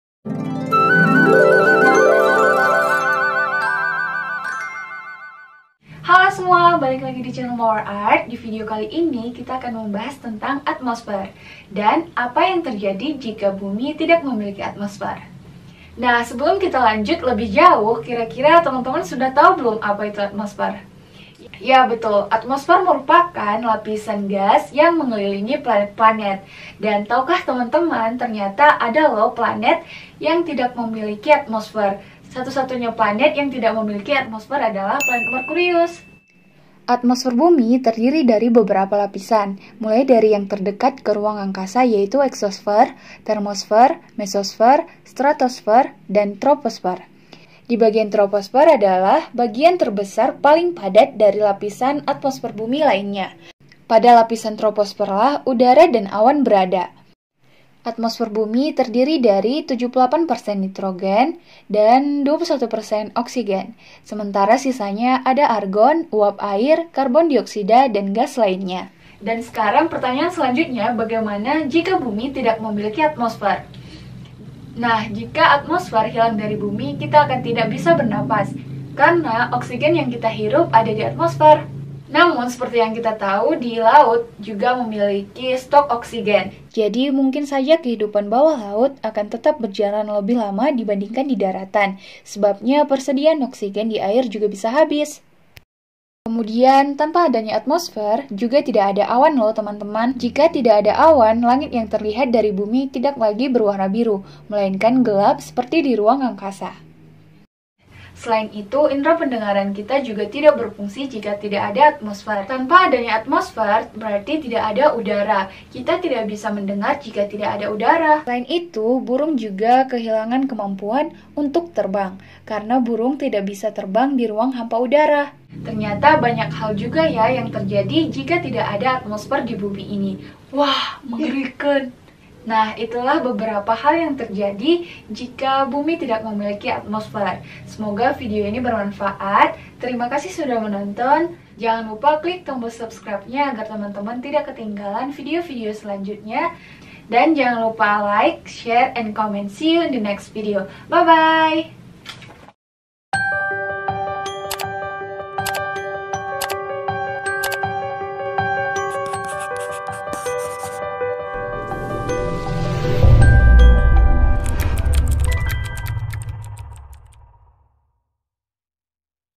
Apa yang terjadi jika Bumi tidak mempunyai Atmosfer???
Atmosfer adalah lapisan gas yang mengelilingi planet.
Bagaimana jika Bumi kehilangan atmosfernya, ya?
Atmosfer Bumi terdiri dari beberapa lapisan, mulai dari yang terdekat ke ruang angkasa, yaitu eksosfer, termosfer, mesosfer, stratosfer, dan troposfer.
Kita tinggal di bagian troposfer, teman-teman. Troposfer merupakan bagian terbesar paling padat dari atmosfer Bumi. Pada lapisan troposfer lah udara dan awan berada.
Atmosfer Bumi paling banyak terdiri dari 78 persen nitrogen dan 21 persen oksigen, sementara sisanya ada argon, uap air, karbon dioksida, dan gas lainnya.
Oksigen yang kita hirup juga ada di atmosfer
Jika tidak ada atmosfer yang mengandung oksigen, maka kita tidak bisa bernapas di daratan.
Namun, di dalam air juga ada oksigen terlarut. Jadi mungkin saja kehidupan bawah laut akan tetap berjalan lebih lama dibandingkan di daratan. Sebabnya, persediaan oksigen di air juga bisa habis.
Kemudian, tanpa adanya atmosfer, juga tidak ada awan, nih.
Jika tidak ada awan, langit yang terlihat dari Bumi tidak lagi berwarna biru, melainkan gelap seperti di ruang angkasa.
Indra pendengaran kita bisa menangkap bunyi karena ada udara. Tanpa adanya atmosfer artinya tidak ada udara juga, dan kita jadi tidak bisa mendengar, deh.
Bukan hanya memengaruhi kemampuan manusia, tidak adanya udara juga memengaruhi kemampuan hewan, yaitu burung.
Sebabnya, burung tidak bisa terbang di ruang hampa udara.
Source : bobo.id